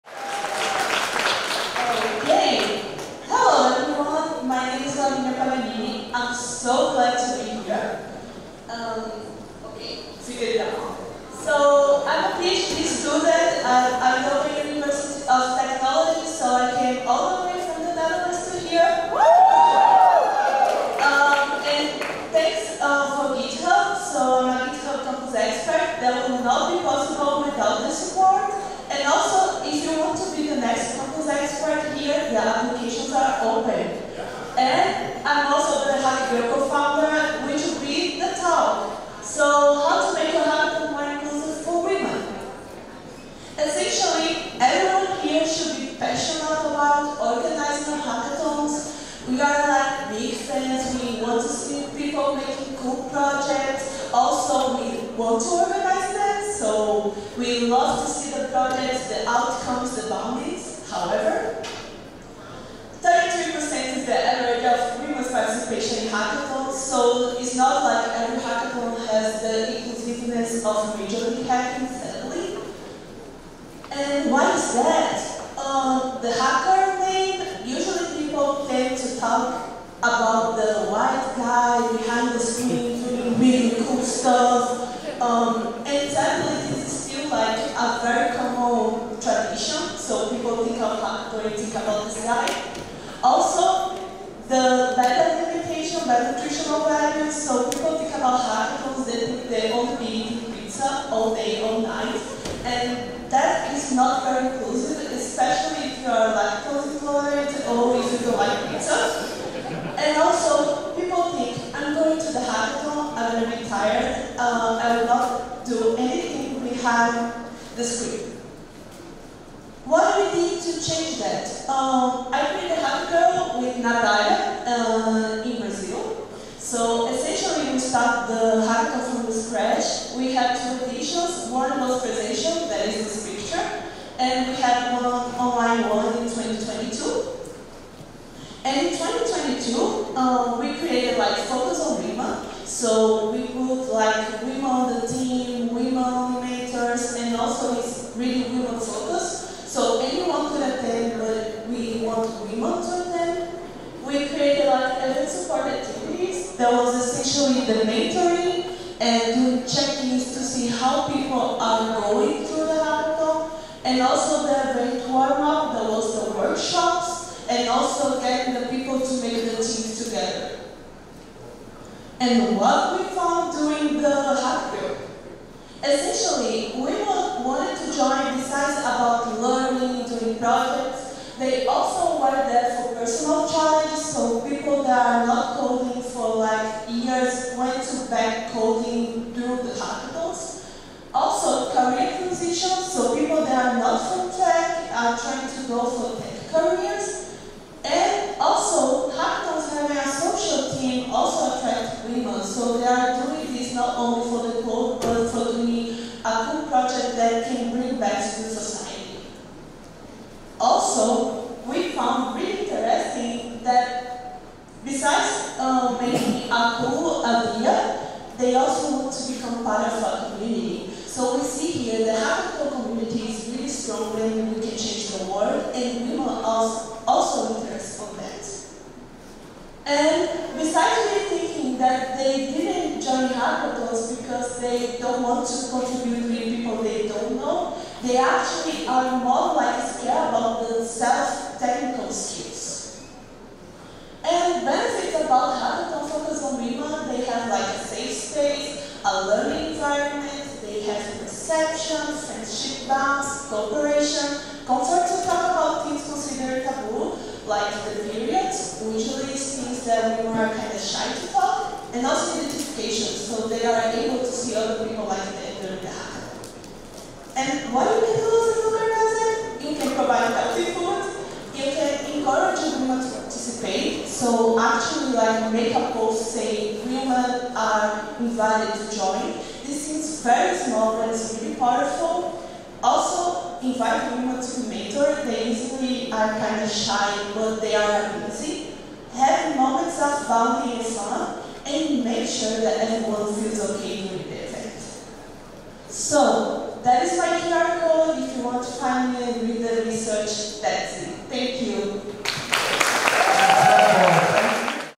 MLH Hackcon XI: Hack GRRRL: How To Make Your Hackathon More Inclusive for Women
This talk is from MLH Hackcon XI, hosted the weekend of August 18th-20th, 2023. MLH Hackcon is an opportunity for hacker community leaders to share stories, tips, and best practices with each other.